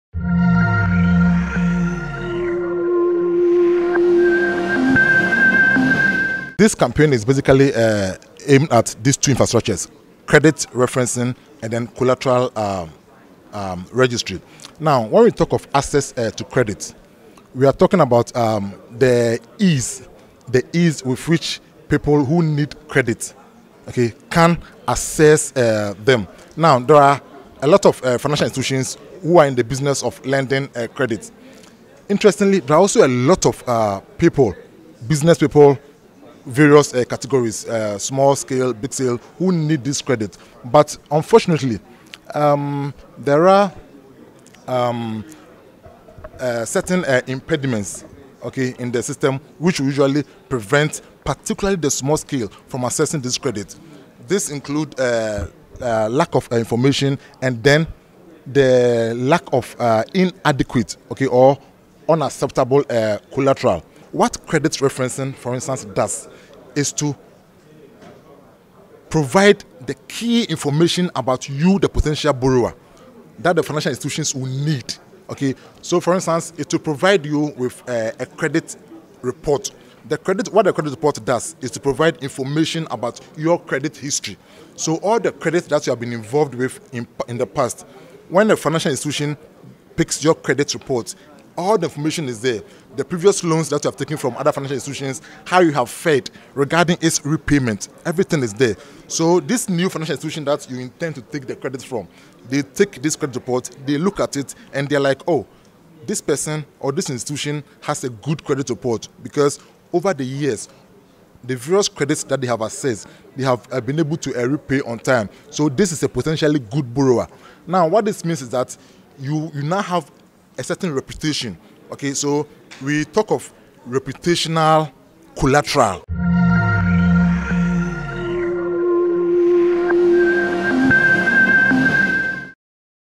Campaign on collateral registry and credit reference bureau system launched
The World Bank in collaboration with the International Finance Cooperation (IFC) and the Bank of Ghana on Wednesday, April 18 launched a public awareness campaign and financial literacy on collateral registry and credit reference bureau system in Accra.